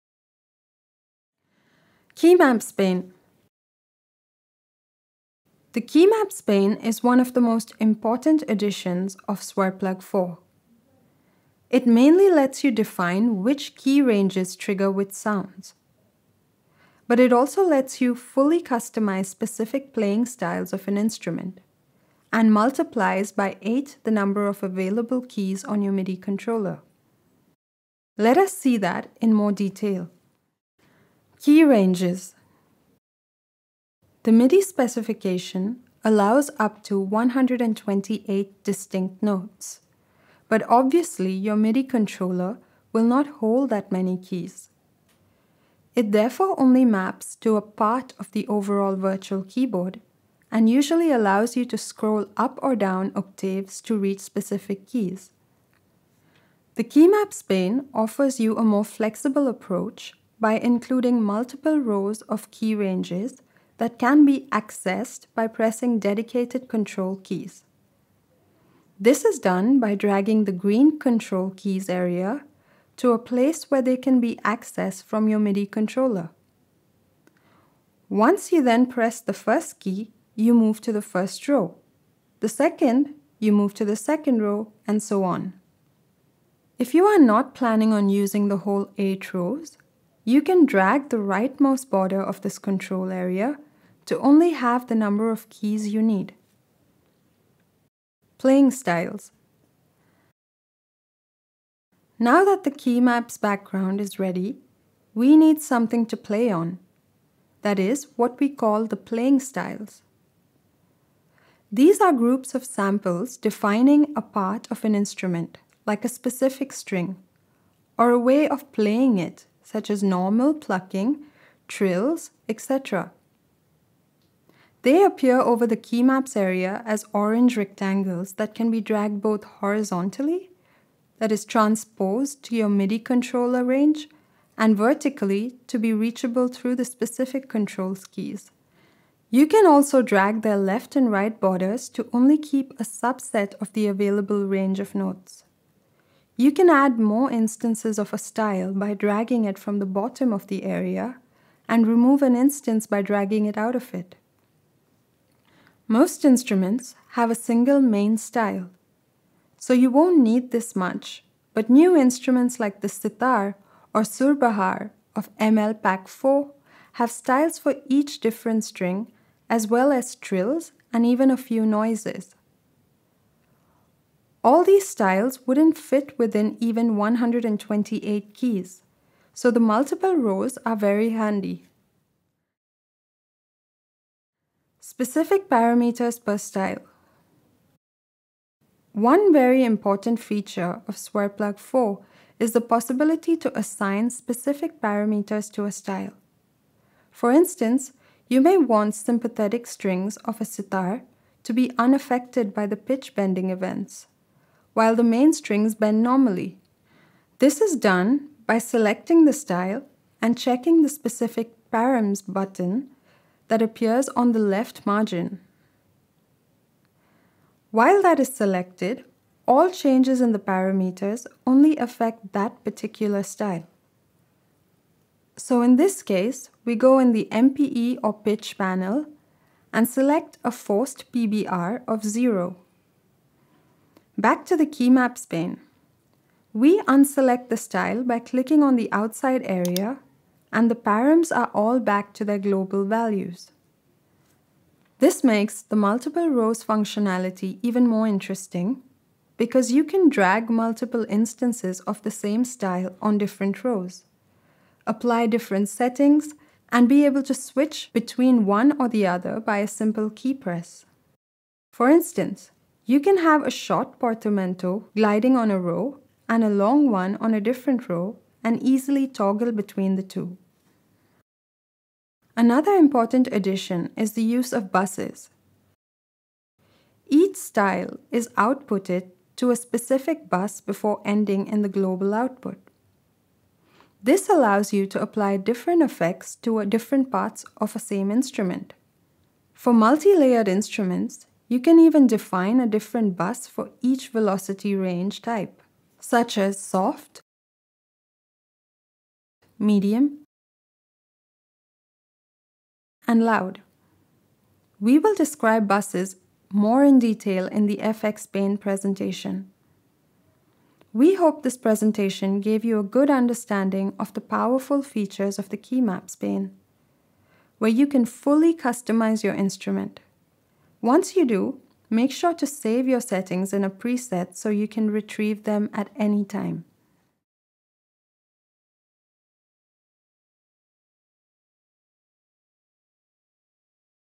SwarPlug 4 VST Plugin: The Keymaps Panel
Discovering SwarPlug 4 music software. This tutorial walks you through the functionality of the SwarPlug 4 Keymaps Pane, where you can customize a selected instrument layout to best adapt to your MIDI controller or playing habits.